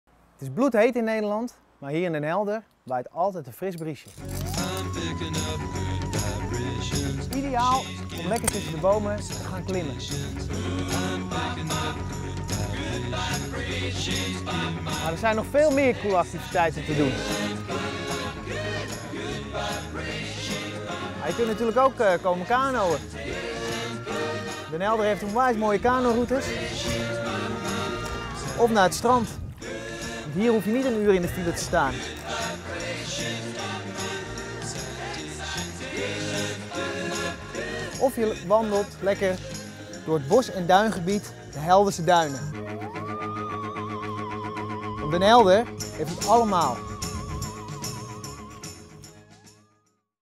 Lekker in de koelte in Den Helder
Heb je het warm? Ga dan eens naar Den Helder. Daar kun je lekker verkoelen.